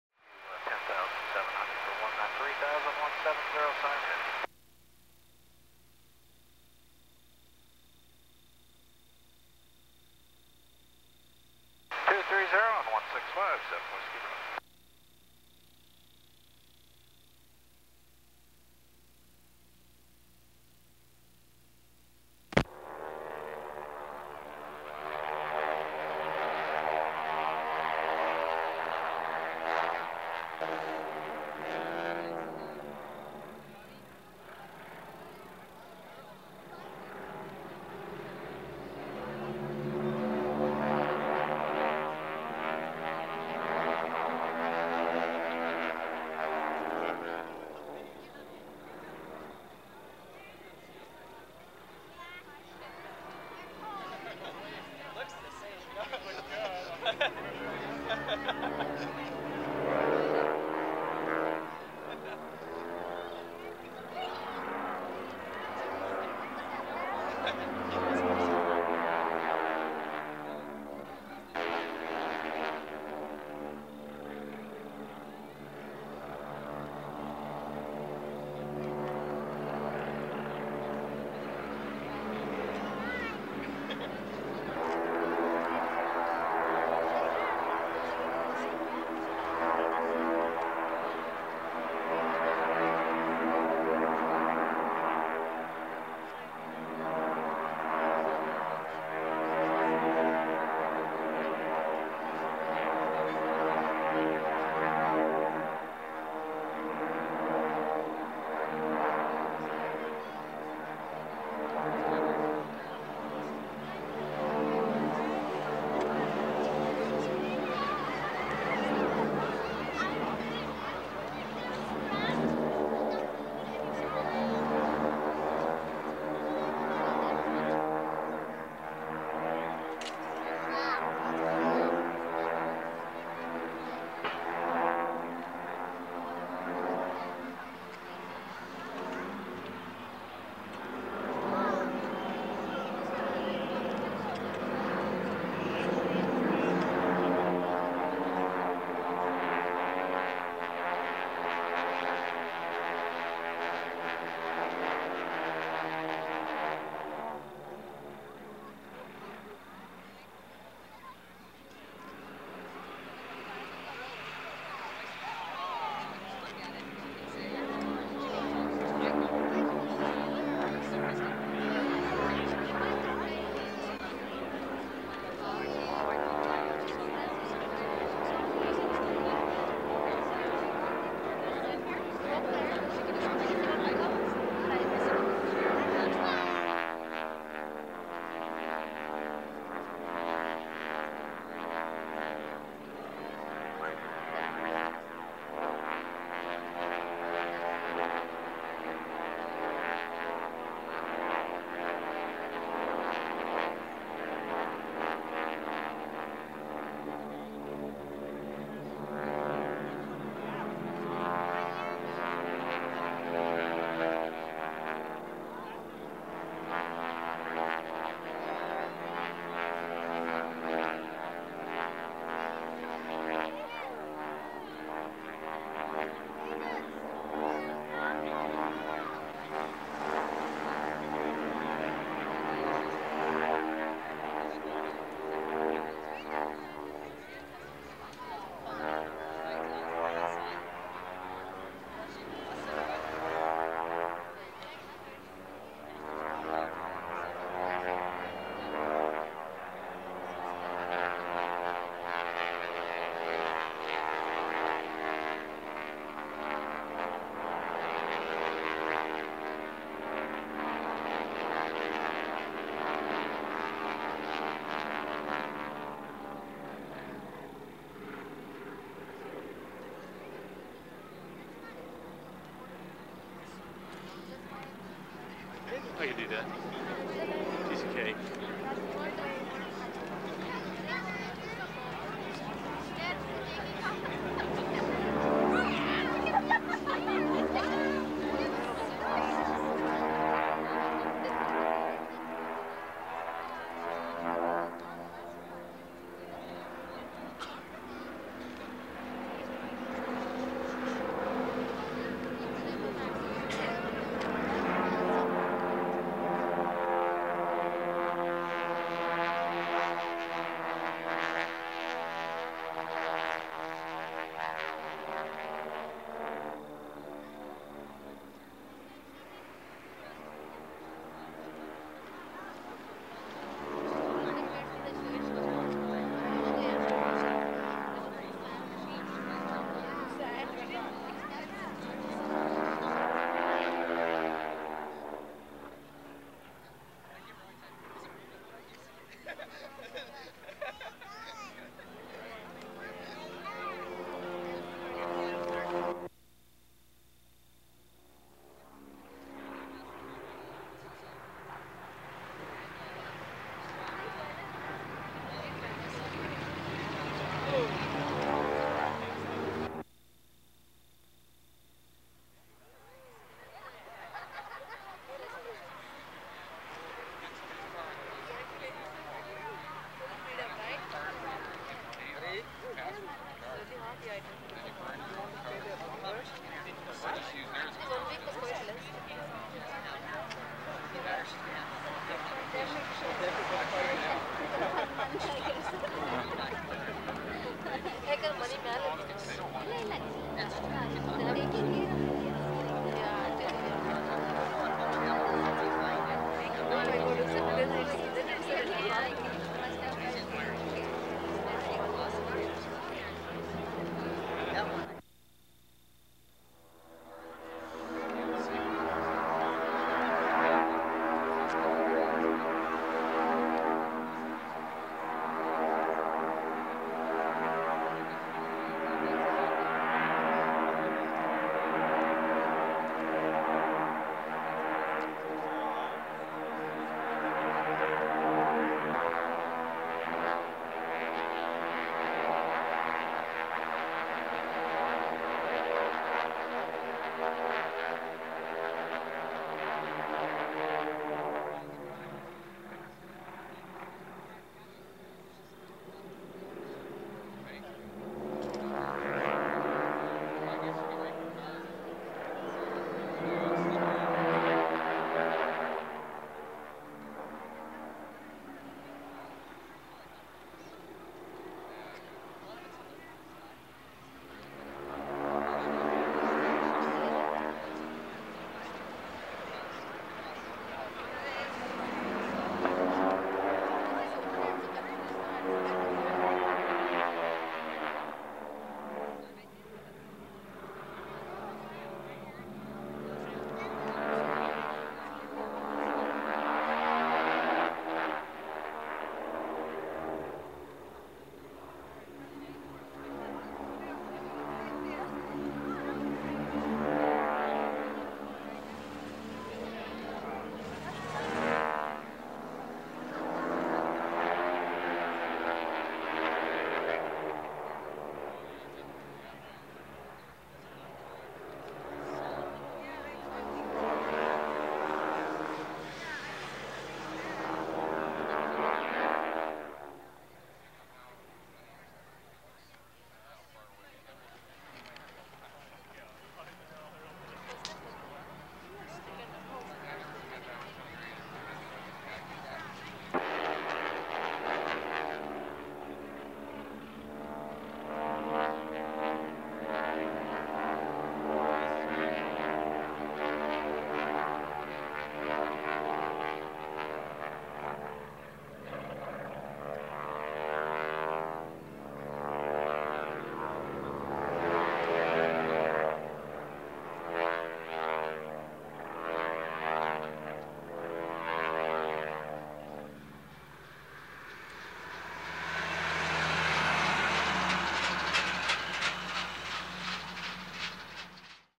Air Show July 12 1996 Red Baron Stearman Squadron Rose Festival
Red Baron Stearman Squadron
The Red Baron Stearman Squadron is proud to recreate a bit of wartime
history as the only formation team flying authentic antique aircraft. The
Stearman model A-75 was built in the late 1930's and 1940's as a trainer
to prepare pilots of the United States Army and Navy for the more advanced
World War II fighter planes. The "Super Stearmans" of the Red Baron
Stearman Squadron have all been restored to a standard configuration and
modified for their current role as airshow airplanes. Few concessions have
been made to change the romantic character of these venerable steel and
fabric birds. There are no radios for navigation on board and both
cockpits are still open to the wind. The pilots of this four plane team,
Bryan Regan, John Bowman, Jim (Sonny) Lovelace, Randy Drake, Steve
Thompson, Tom Womack, Travis Aukes, Marv Randall, Steve Elm and Randy
Brooks are supported by narrators Jerry and Margaret Van Kempen and a
staff of skillful mechanics.